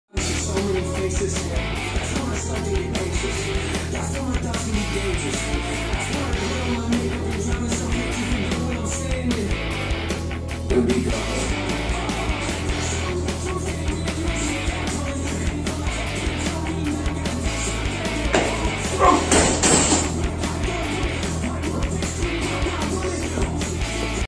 Just an update. Last video removed due to copyright claim 🤣👍🏻
Same video but added music.  Last one got claimed 🤣.

Anyway, here is me testing 140kg clean and failing. Almost got it tho 😎👍🏻